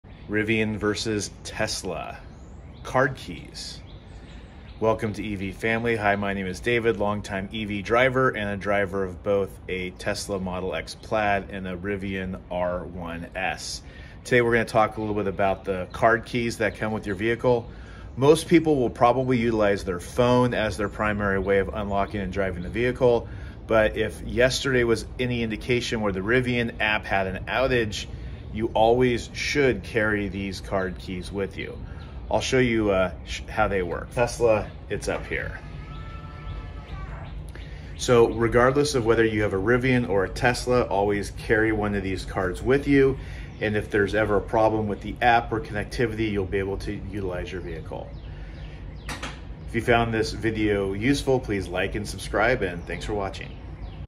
Rivian R1S vs Tesla MX Plaid - Card Keys - Must Carry These!!
Rivian R1S and Tesla Model X Plaid key cards - you should carry these